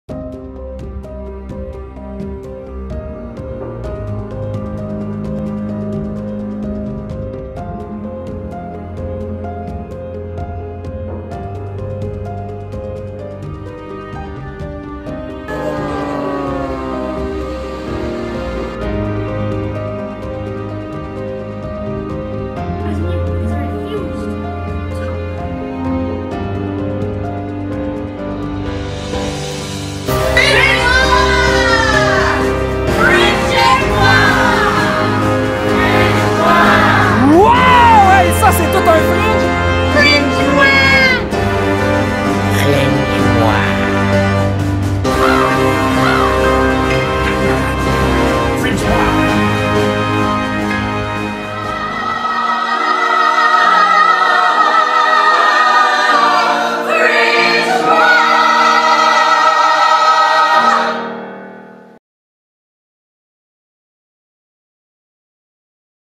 FRINGE-MOI MINI COLLAGE: PRE-FESTIVAL
Le FRINGE s'en vient! Ce FRINGE-Moi Mini collage est une compilation de vidéos filmés par les artistes et les employés du Festival St-Ambroise FRINGE de Montréal. Vous êtes tous et toutes invités à participer en déposant vos vidéos sur Twitter ou Facebook avec Fringemoi et fringebuzz

GET PUMPED! This FRINGE-Moi Mini collage is a compilation of videos taken by artists and staff of the St-Ambroise Montreal FRINGE Festival. Anyone is welcome to participate by posting videos to Twitter or Facebook with fringemoi &

Artistes dans le vidéo / ARTISTS in the video
00:00 & 00:35 Serge Yvan Bourque
00:11 & 00:47 Chocolate Moose Theatre
00:15 & 00:39 Possiblement Théâtre
0:18 & 0:33 d² Productions Montréal
00:22 Nisha Coleman
00:42 32-Hour Perspectives
0:44 Cimonac Productions
0:52 Kaleidoscope Theatre Montreal